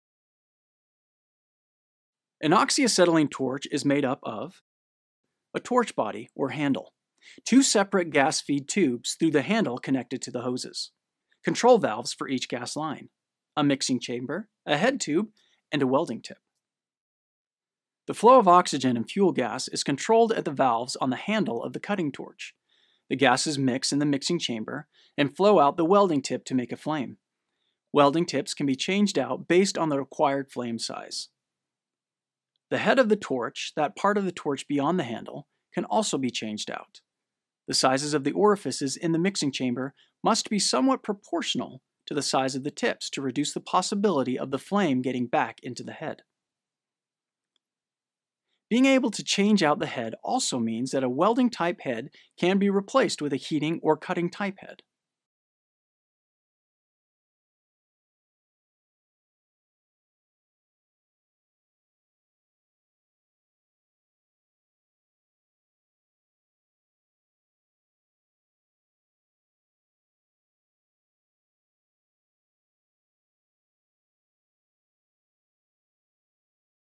Oxyacetylene Welding Equipment and Safety
to view the full video and purchase access to our other Industrial Maintenance courses.

Oxyacetylene welding, also known as gas welding, is a process which relies on the combustion of oxygen and acetylene to produce a very hot flame. When these gases are mixed together in the correct proportions, a flame is produced with a temperature that is sufficient to melt steel. This course will cover the basics of oxyacetylene welding and some best practices that should be followed in order to be safe on the job. We will go over how oxygen and acetylene are used in gas welding, the equipment that makes up a gas welding rig, and the PPE required to maintain a safe welding environment.